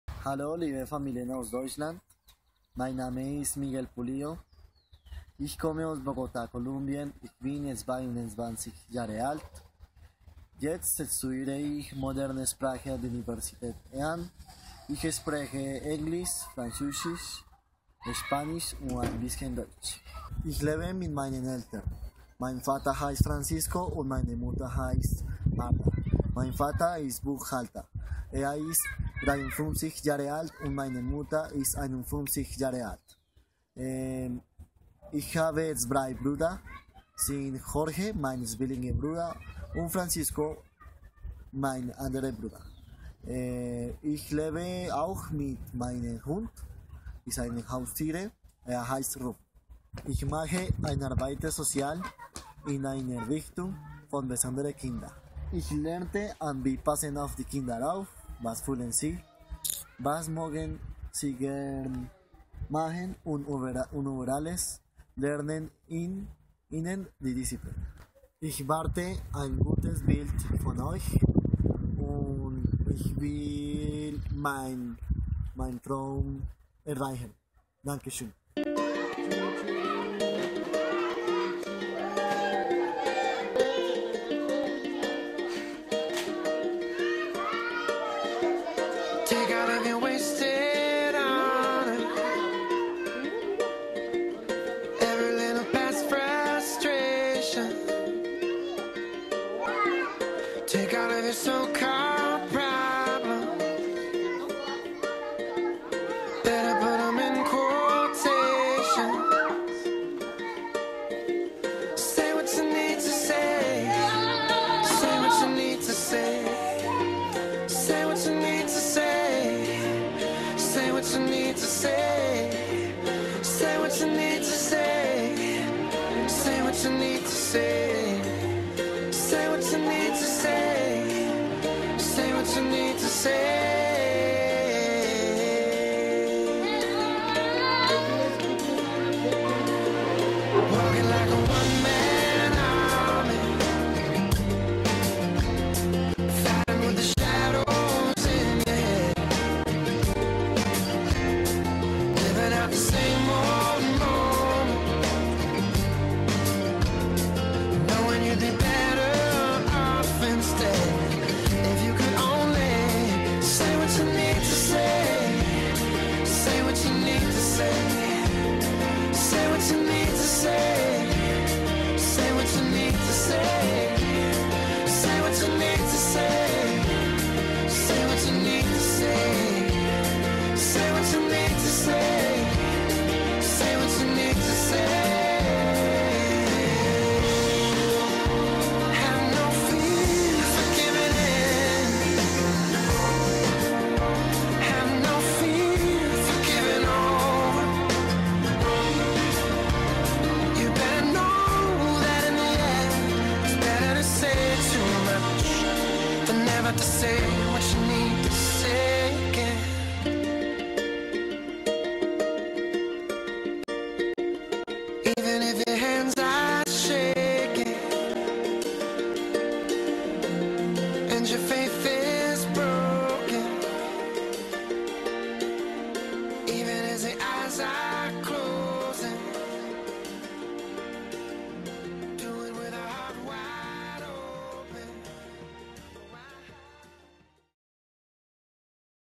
Meine Vorstellung bei euch, Familien aus Deutschland. Viele Grüsse Miguel Angel Pulido Lizcano
Das ist meine Vorstellung bei euch, ich will euch zeigen: Warum will ich Au pair sein.

Ich bin ein junger Mann aus Kolumbien.